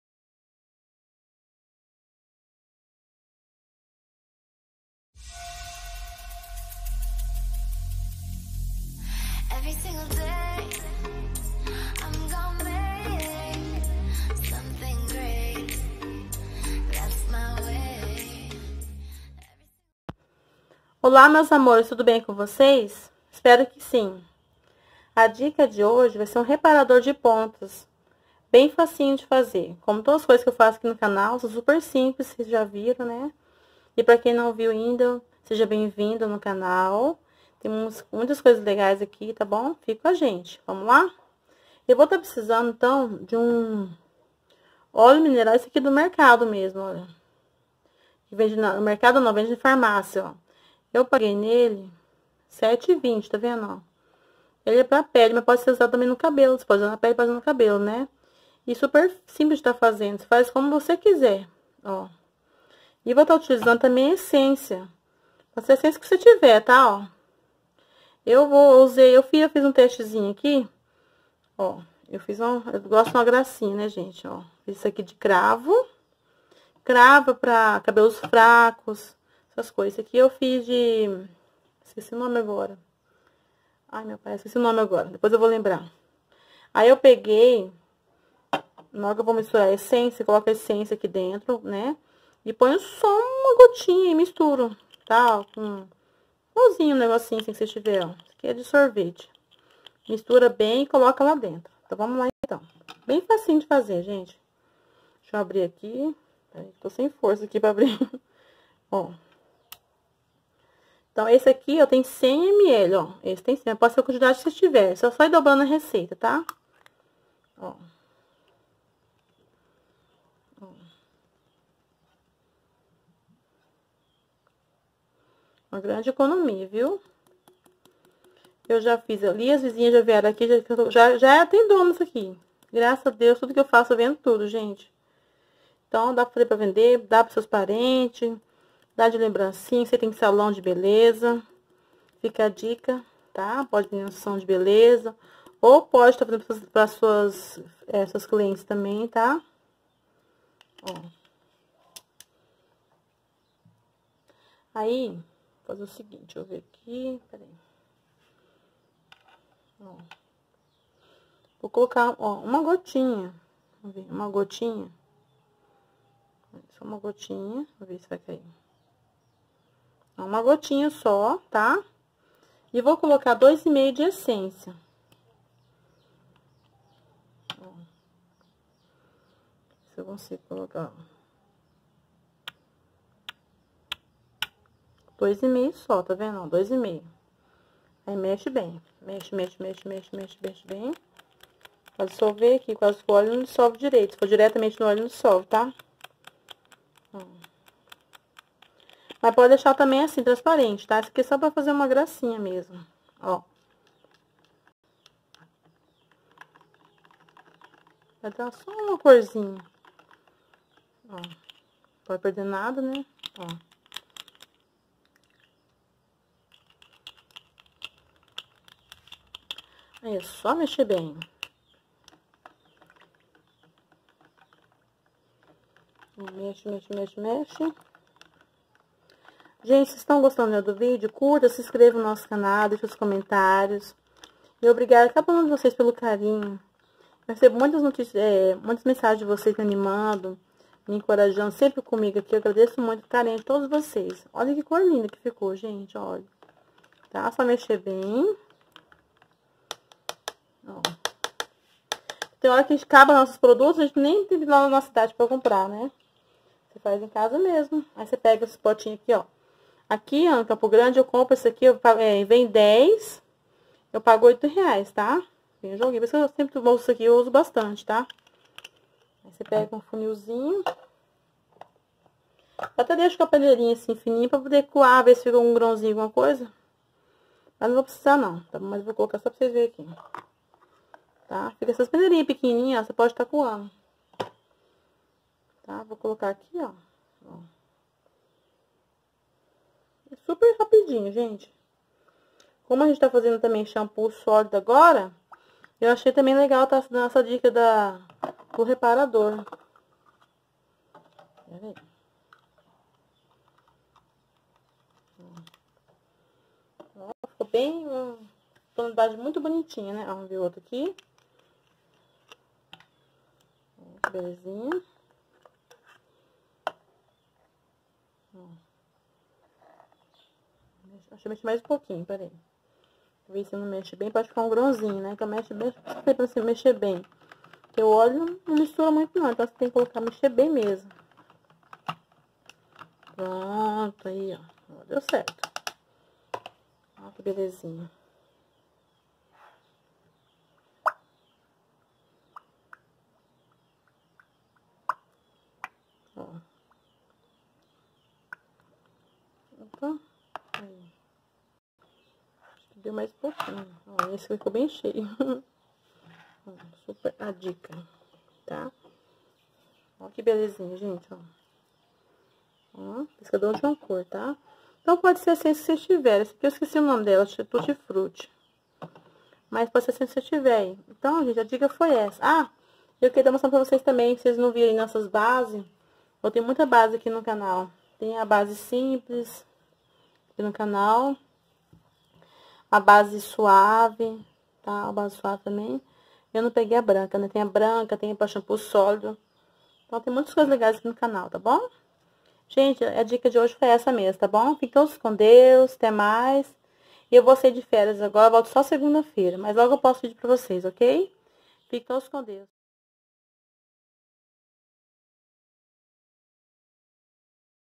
Como fazer Reparador de Pontas..Brilho Intenso e Maciez
Ingredientes 💞

1 vidro de óleo mineral (farmácia)
2,5 ml de essência
Corante(opcional)

Assista o vídeo para ver passo a passo

🌺O óleo mineral hidrata cabelo seco..bom pra queda..da leveza nos fios.. proporciona brilho🌺

Como fazer Sabonete Masculino Senador

Curtam. Se inscrevam..comentem..
A todos obrigada pelo carinho 💞

Se copiarem Deixem os créditos ao canal Márcia Jesus Dicas 🌺🌿🌺